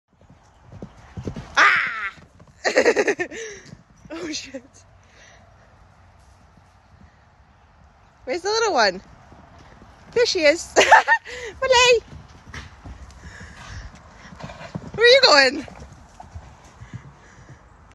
Grace scares her horse Sven, he lets out a wee toot.
Sven loving his life. Molly and Mel too.